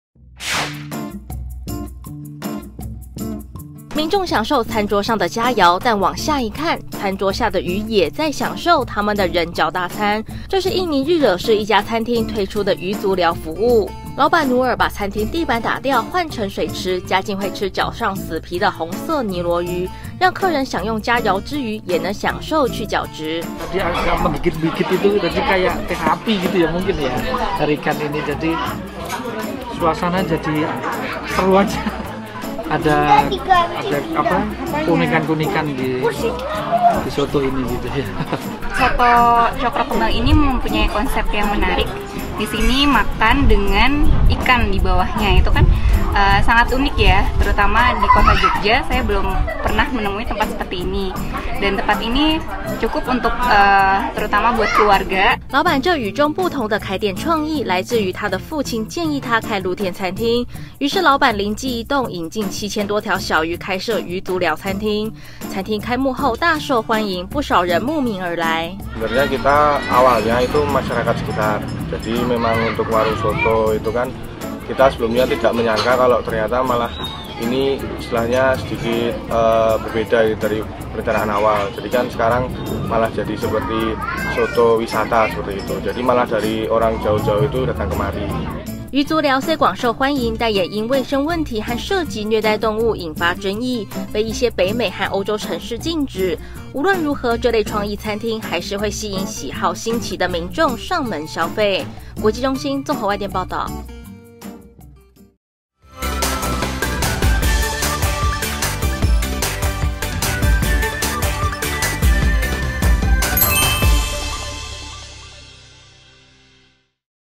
顧客到餐廳吃飯同時被魚吃 印尼餐廳用這噱頭攬客 | 台灣新聞 Taiwan 蘋果新聞網
印尼日惹市一家餐廳推出魚足療服務，讓顧客一邊享用餐點，一邊被魚咬。

老闆努爾把餐廳地板打掉換成水池，再放入吃腳死皮的紅尼羅魚。讓客人享用佳餚之餘，也能享受腳部去角質。這項服務推出後，受到大眾歡迎。顧客認為這家餐廳的概念非常有趣，而且餐廳夠大，可以帶全家大小一起來。也有人認為雖然被魚咬腳很癢，但很有趣。